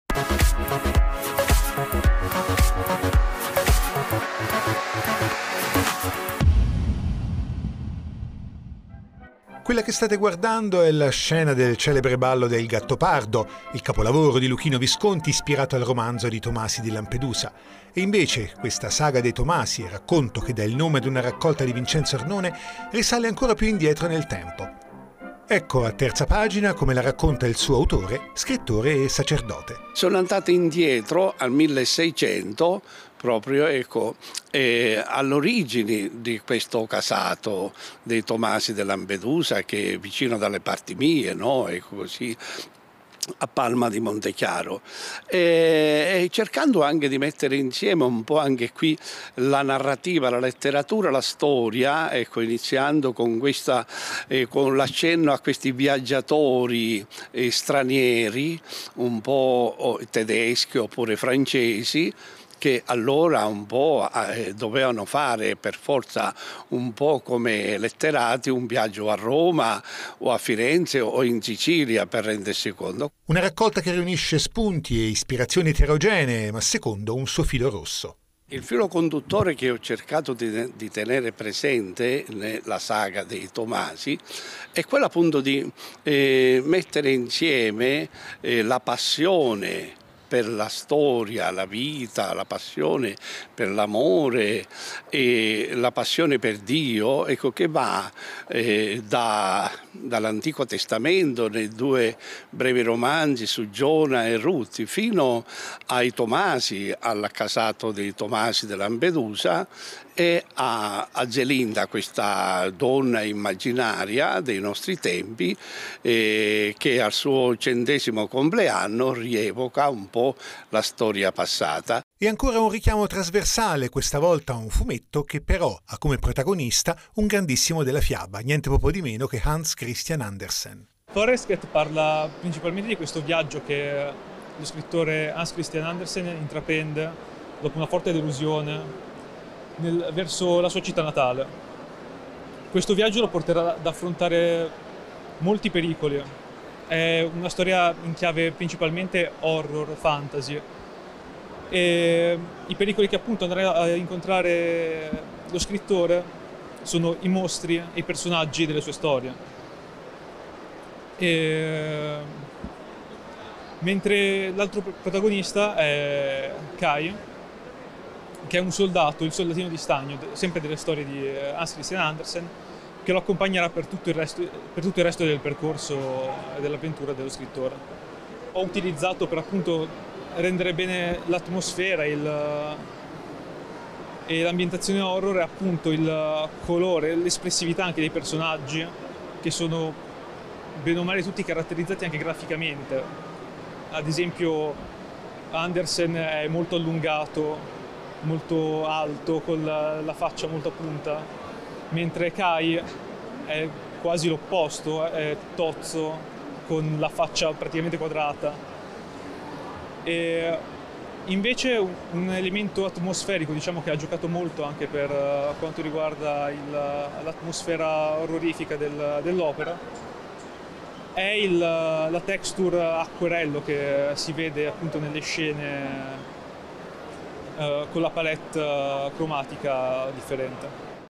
A Terza pagina il libro 'La saga dei Tomasi' di don Vincenzo Arnone e il fumetto 'Forelsket'
Al centro della puntata il libro 'La saga dei Tomasi' di don Vincenzo Arnone e il fumetto 'Forelsket' di Matteo Filippi e Lorenzo Ridolfiche con protagonista un grande della fiaba: Hans Christian Andersen.

 A cura di Saverio Simonelli.